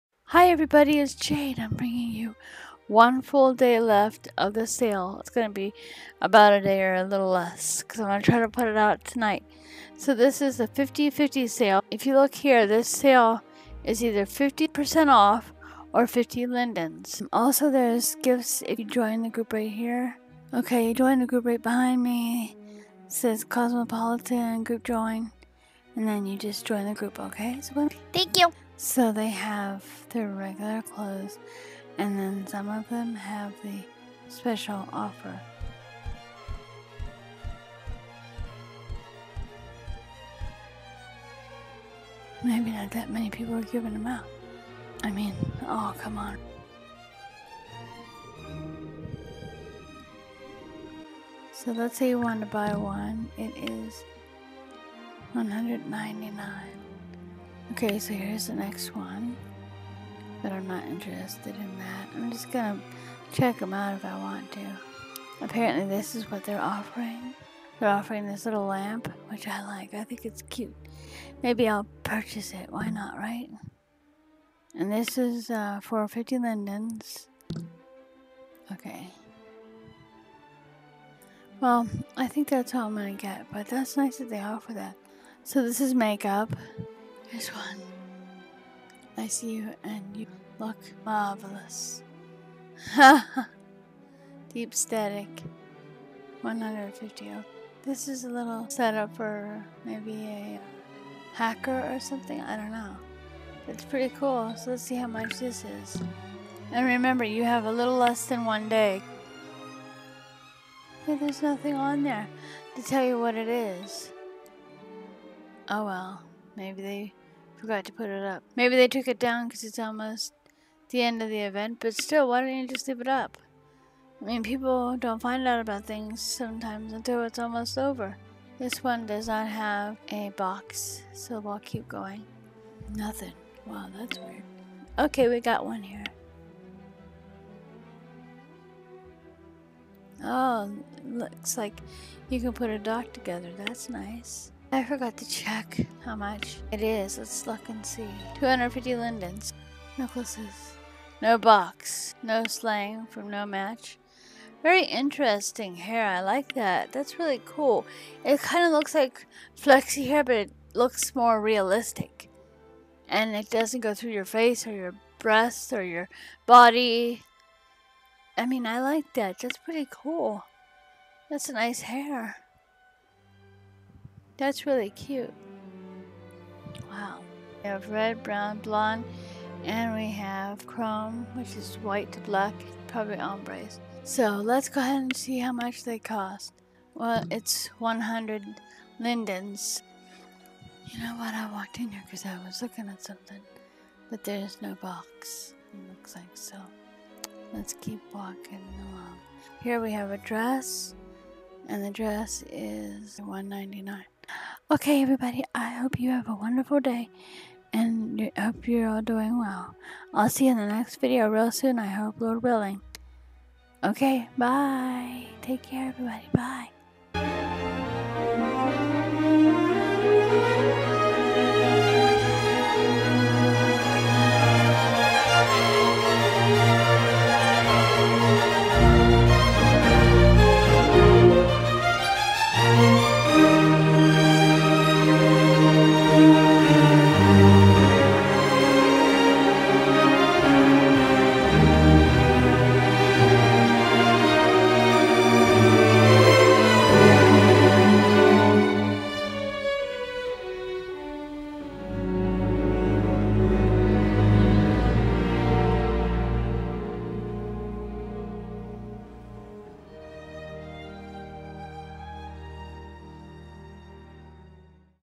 Cosmopolitan Sale 50 % off/ 50Ls
All the creators who were giving out things for the sale seem to be downstairs. :)
Huggies, Jaide Mwaaahhh. :D